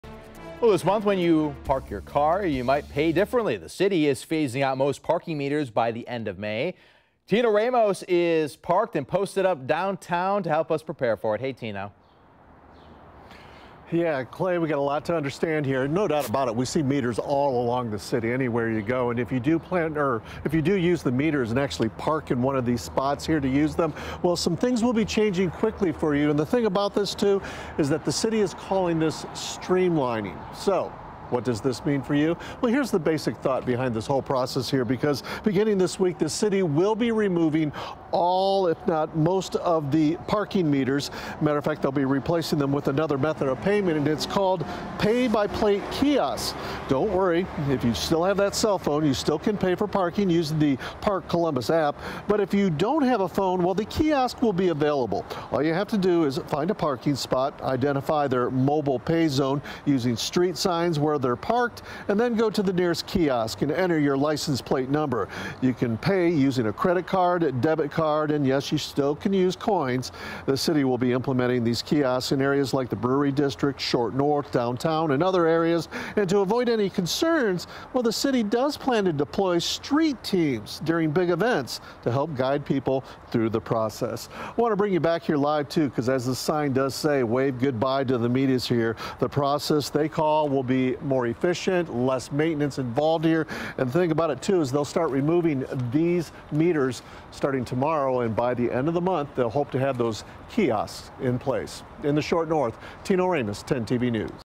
Columbus to start replacing parking meters with new pay-by-plate kiosks in May
The city says this will modernize and streamline the payment process.